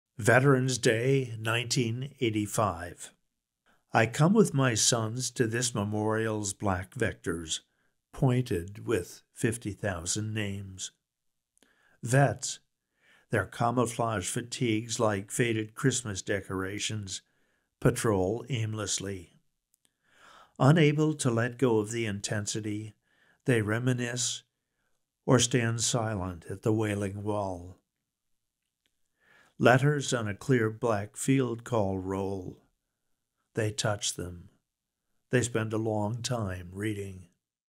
Veteran's Day, 1985
A poem about visiting the wall of names as a Vietnam veteran.

Sign up to receive John Barr poems and brief thoughts about poetry for free